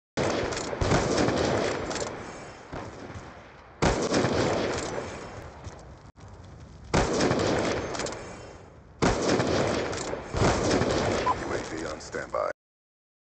FFA Ironsight Clip! (Leftover)
Hey guys Viper here and to day I'm bringing you this little lefty ironsight I hit in FFA.